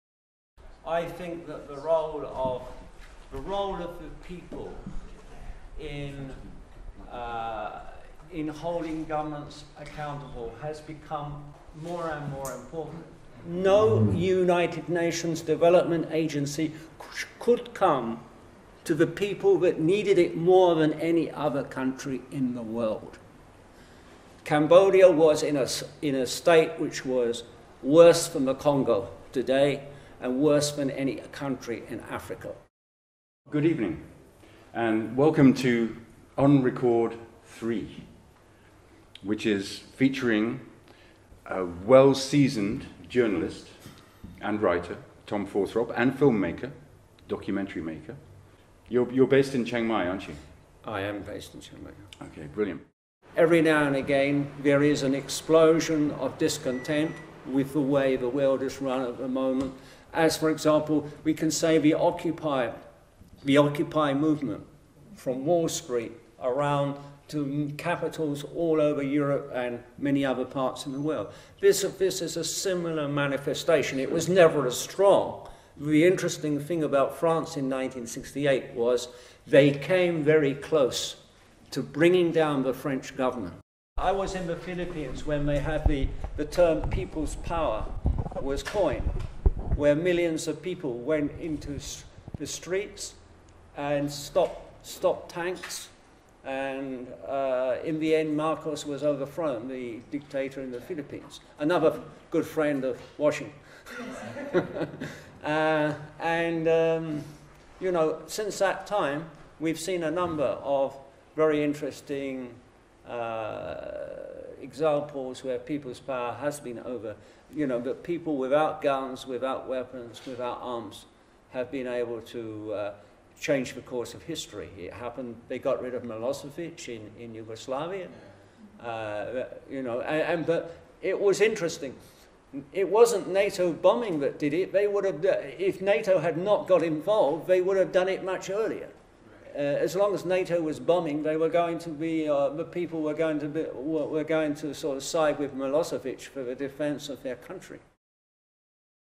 On Record 3 Part 1 Tom Fawthrop - Intro
An interview with English journalist and film maker, Tom Fawthrop.
Recorded at Sangdee Gallery in Chiang Mai, Thailand as part of
a series of interviews called 'On Record'.